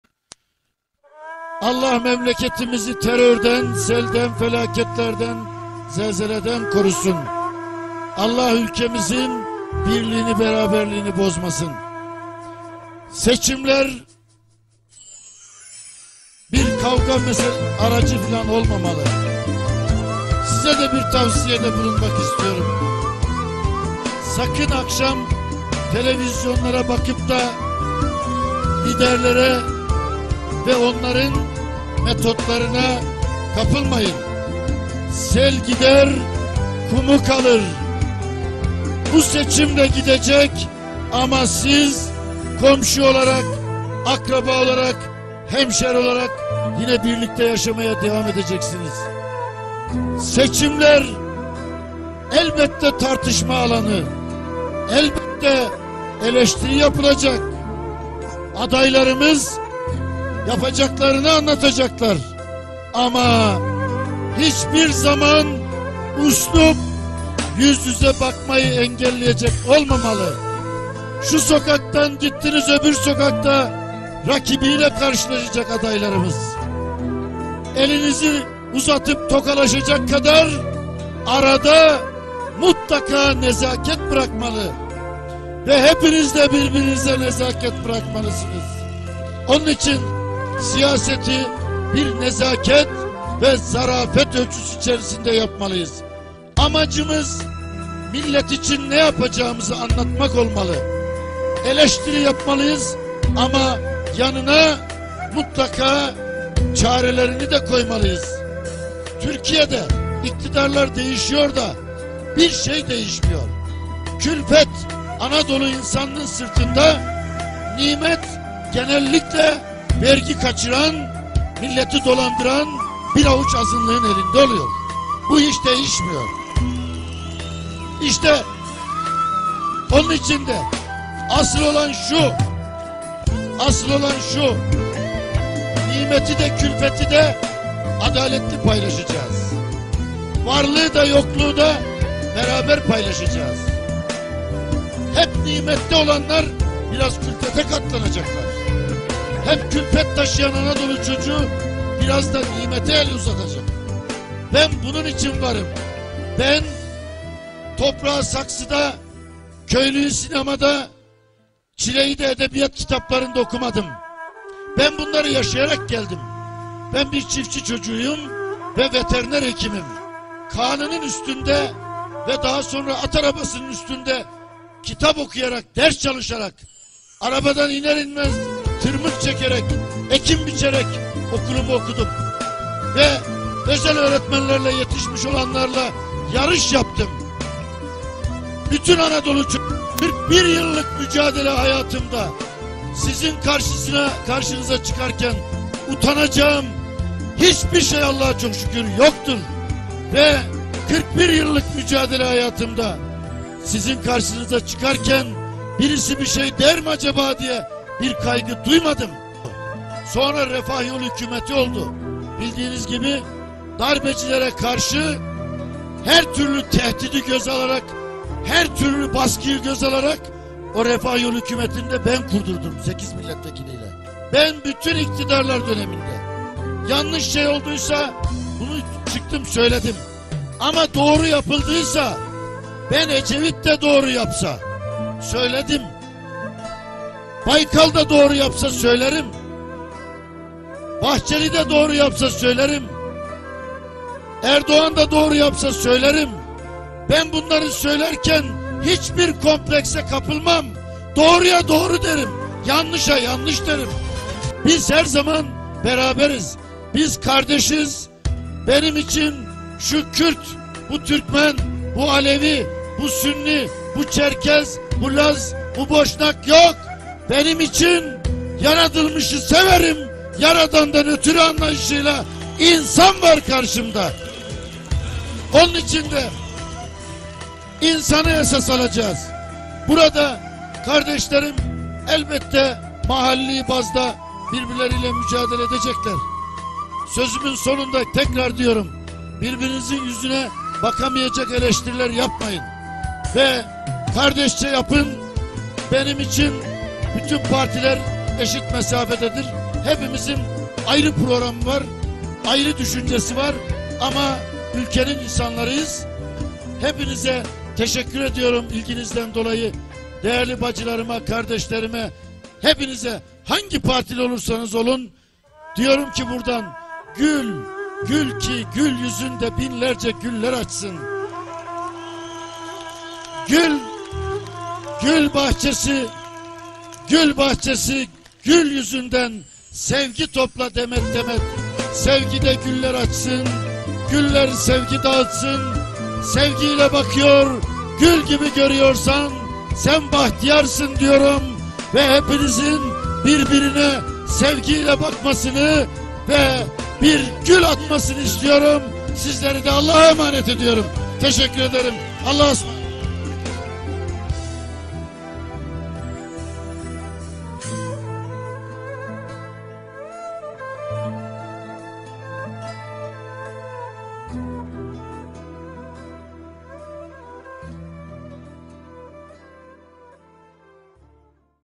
Merhum Muhsin Yazıcıoğlu'nun vefatından önceki Son Mitinginde Aziz Milletimize Son Nasihatleri
Sivaslı Bir Hemşehrisi Olarak;
Örnek Bir Dava Adamı Olan, Şehid Muhsin Yazıcıoğlu'nu Rahmet ve Şükranla Anıyorum.
İsmail Pendik